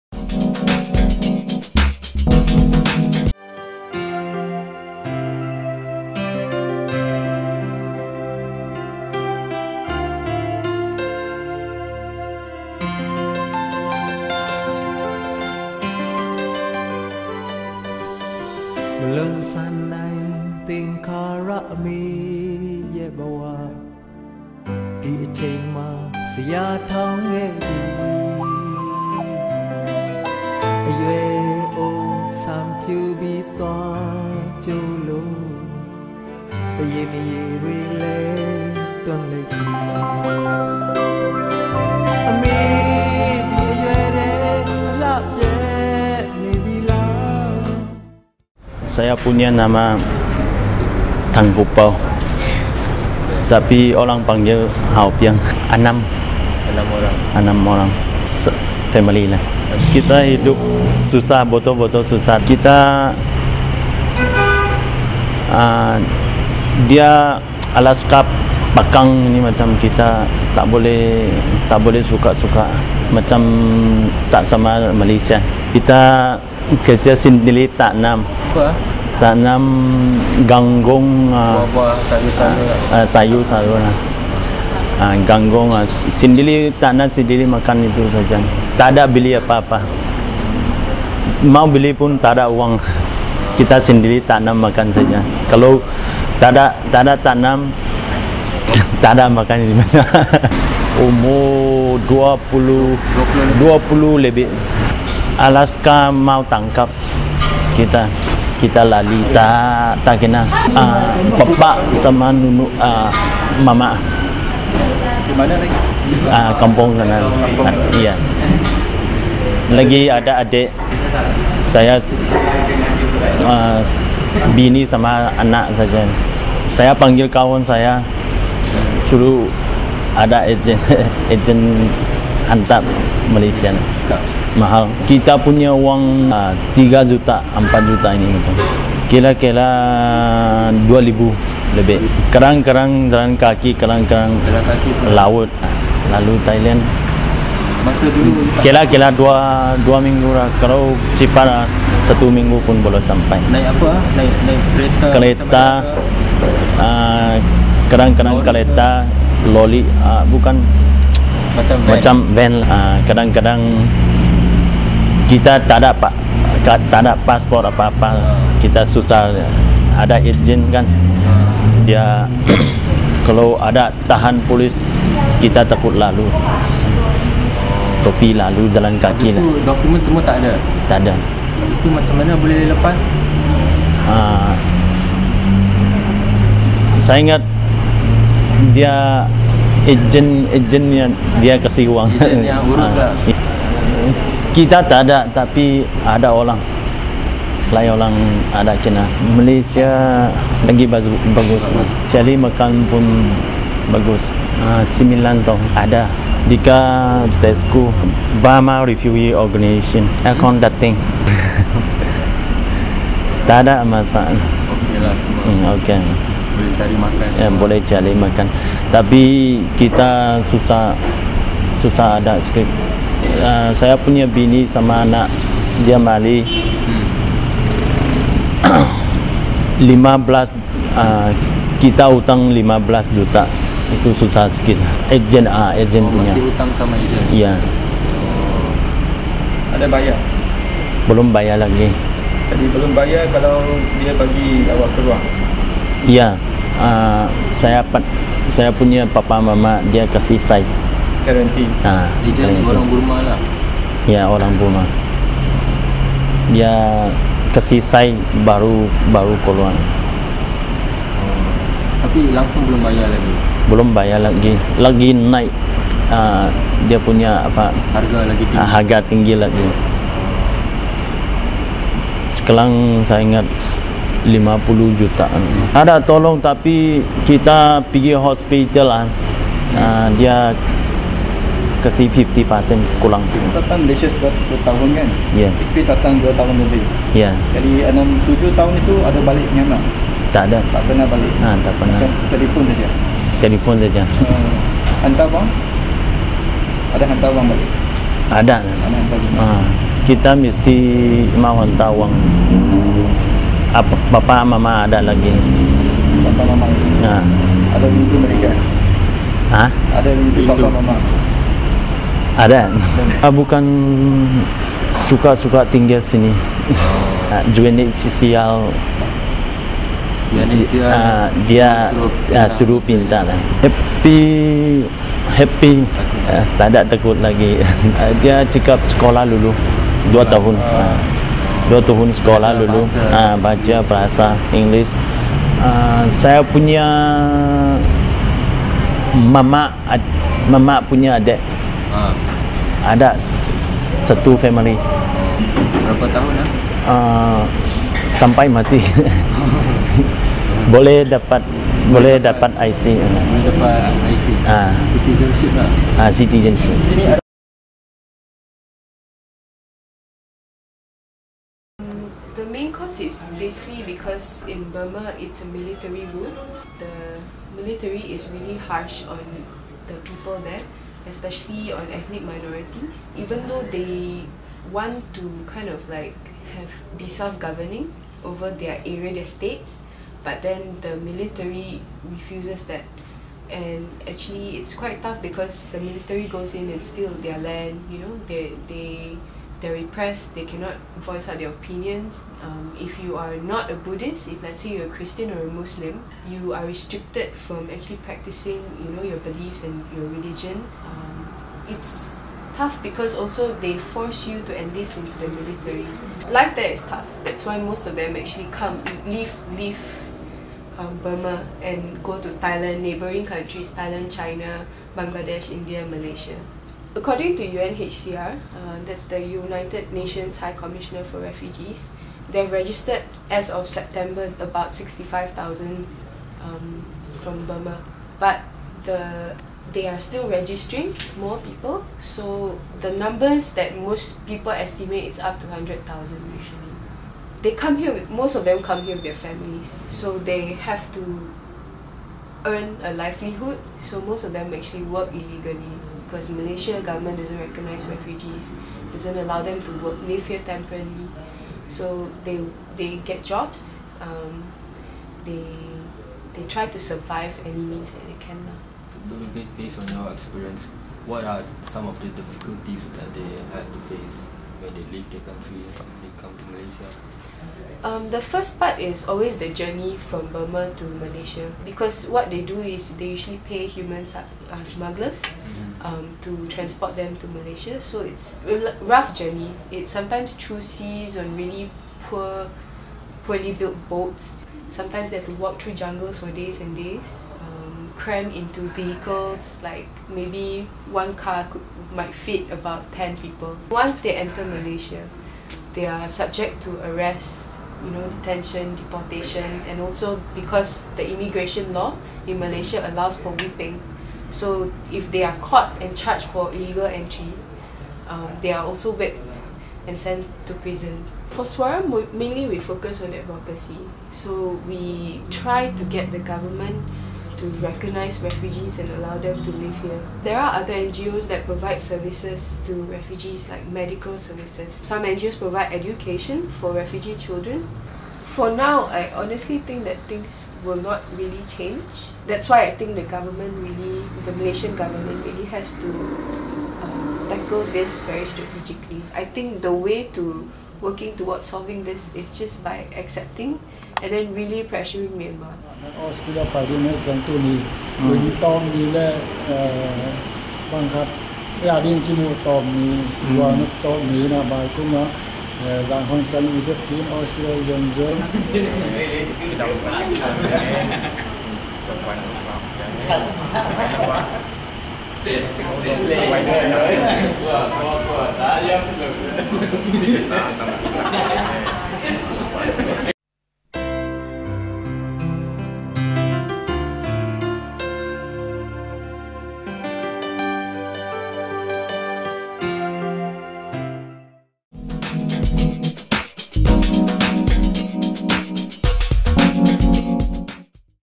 Life and Times of Burmese Refugee Families in Malaysia
Unwanted and unwelcomed by their own government and often being subjected to constant harassment, the Burmese ethnic Karen minorities were forced to flee their village and braved the jungles to escape into neighbouring countries such as Thailand and Malaysia.